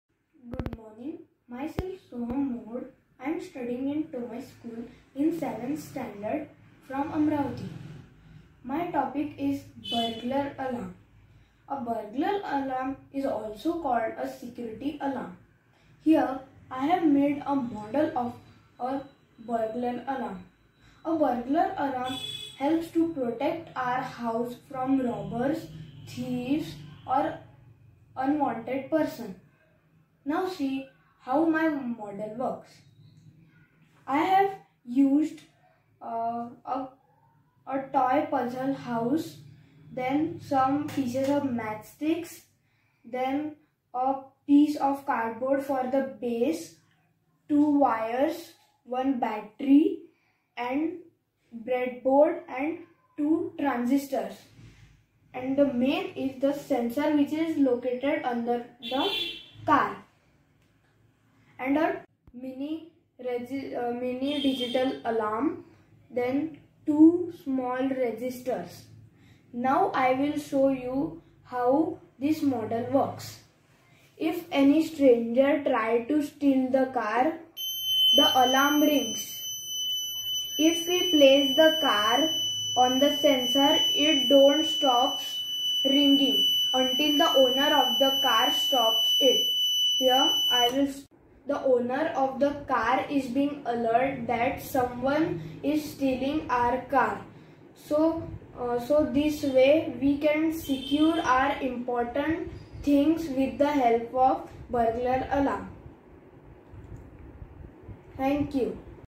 Burglar alarm
Material used

breadboard
baterry
transistors
resistors
small nut bolts
mini toy car
a sensor
peice of cardboard for base
paper house used in puzzle